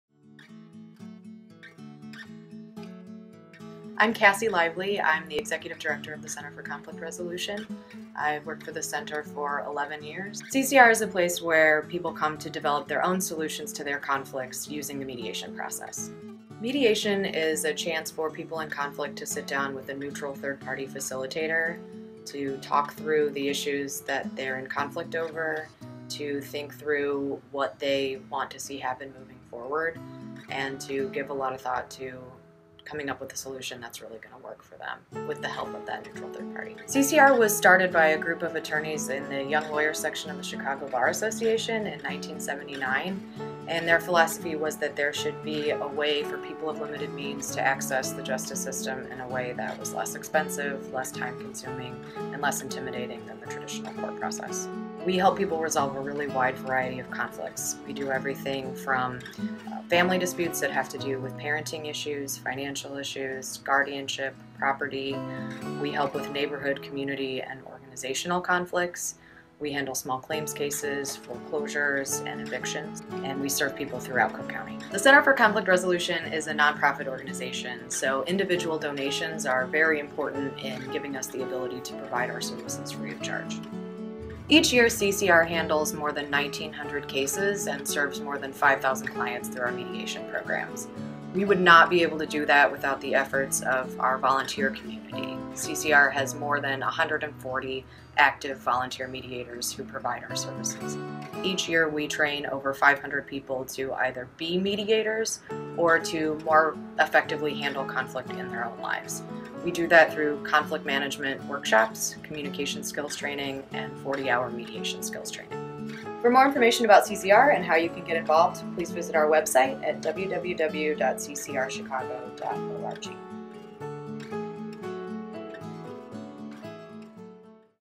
The Center for Conflict Resolution - Intro Video
The Center for Conflict Resolution (CCR) is an independent, not-for-profit organization with a mission to work with individuals, communities, courts and other institutions to manage and resolve conflict. Since 1979, CCR has accomplished this mission by offering pro bono mediation services and conflict management training to Chicago area institutions, organizations and businesses.

Video edited by Megan Rogers-Reilley